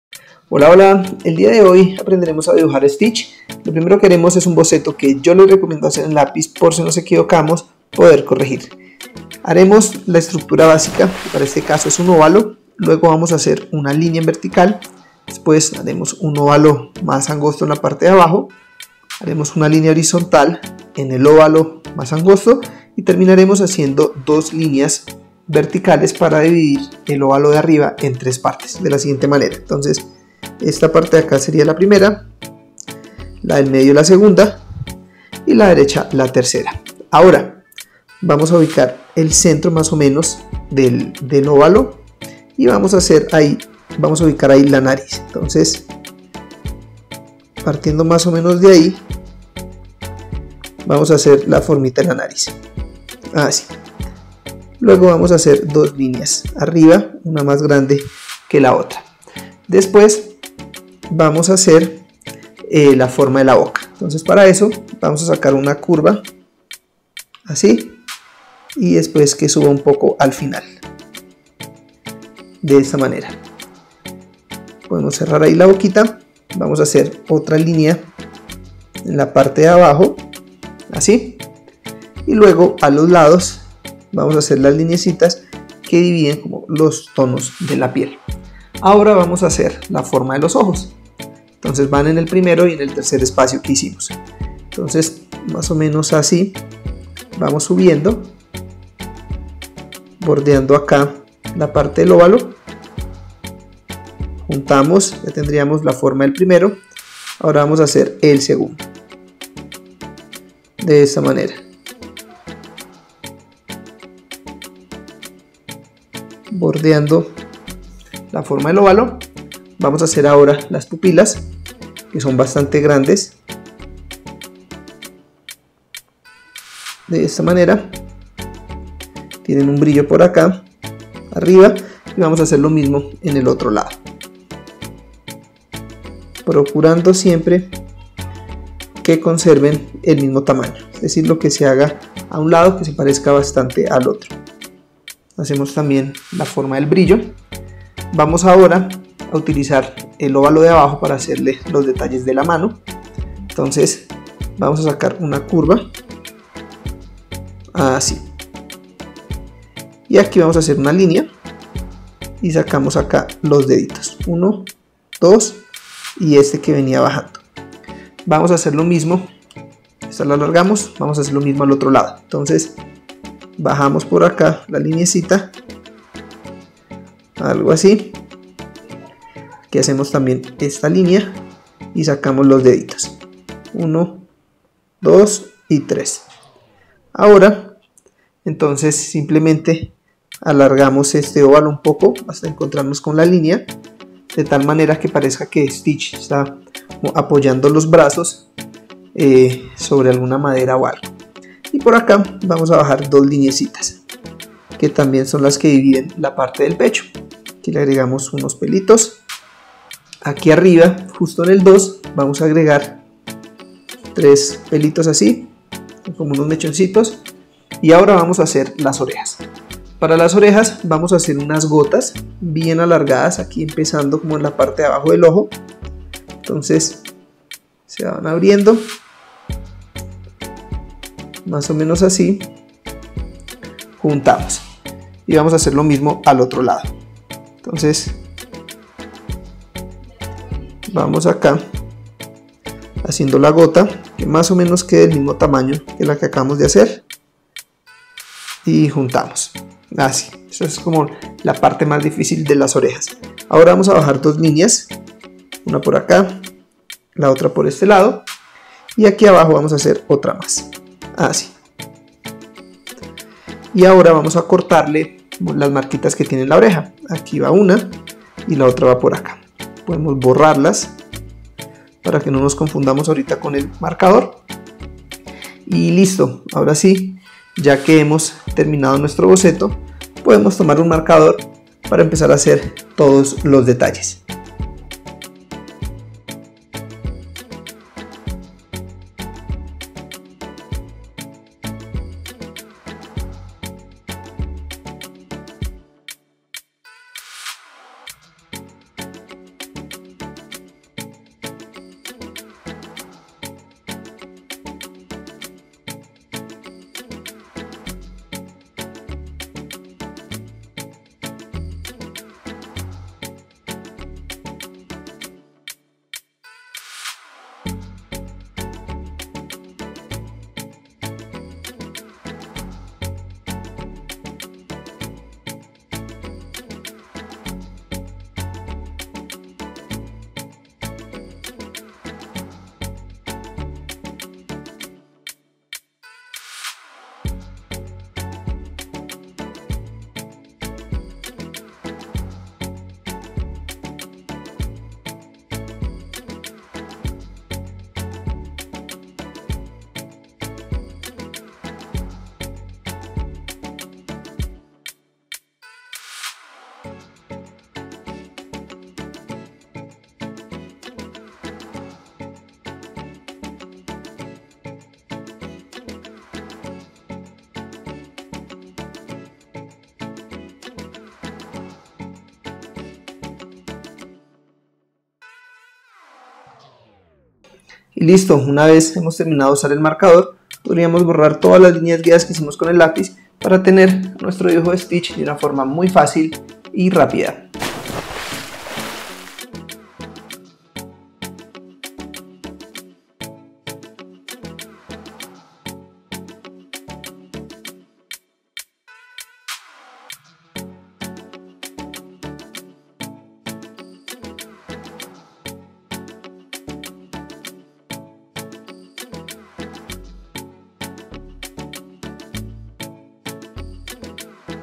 Cómo dibujar a Stitch paso a paso | Dibujos fáciles
Aprende cómo dibujar a stitch paso a paso y de la forma más fácil. Tutoriales de dibujo faciles para que aprendas a dibujar dibujos fáciles.